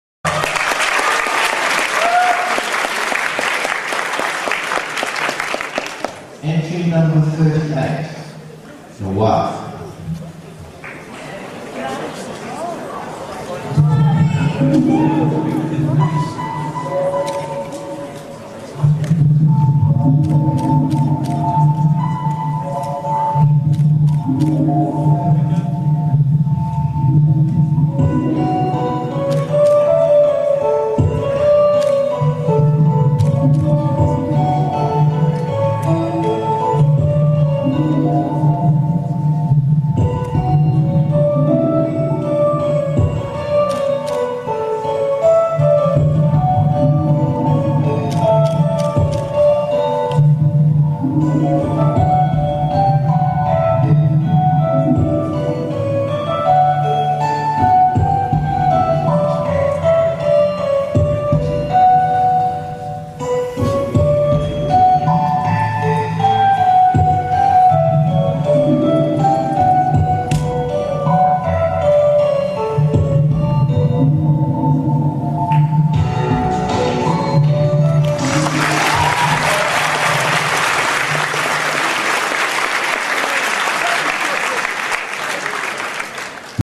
AX New York 2002 - #38 Chloe
Video of Entry 38, skit of Chloe from Noir performed at Anime Expo New York 2002 Masquerade.  (Permission granted)
(Won Darryl Hwang's Judges Award Craftsmanship)